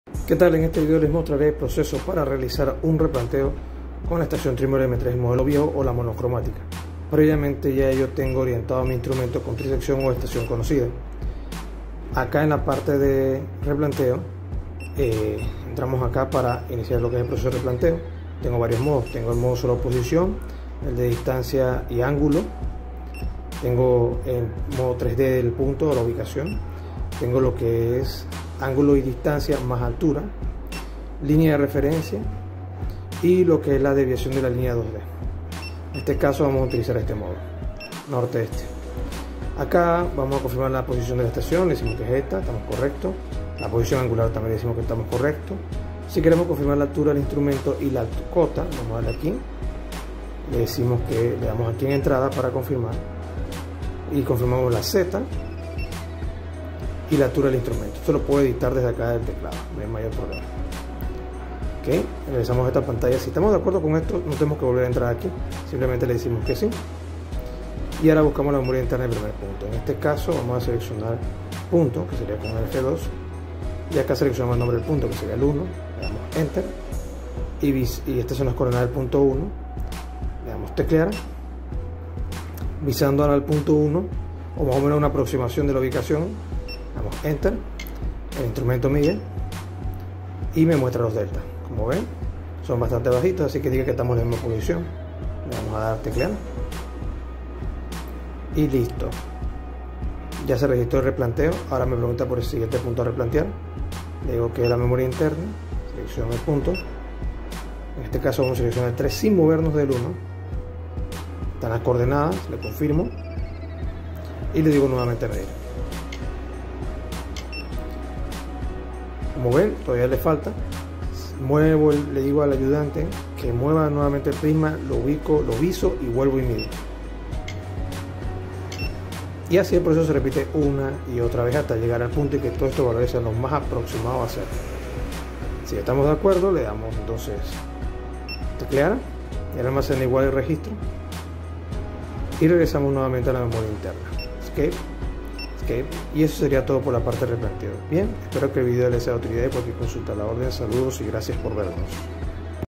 Trimble M3 (modelo viejo, pantalla monocromática) parte 5: replanteo
En este vídeo el cual es parte de unas serie de vídeo les mostrar la operación básica de la Trimble M3 modelo viejo o de pantalla monocromática, en este caso para lo que es el proceso de replanteo

Aquí dejare el siguiente link para que puedan ir a una playlist donde tengo otros vídeos relacionados con ese tema.

Cualquier duda a la orden, saludos y gracias por vernos.